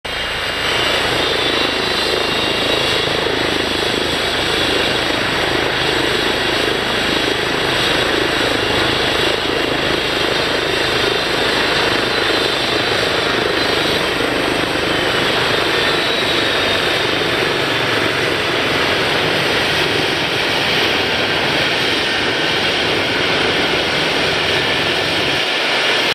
Meeting Aérien de la BA 116
Entre hélicoptère de Gendarmerie et militaire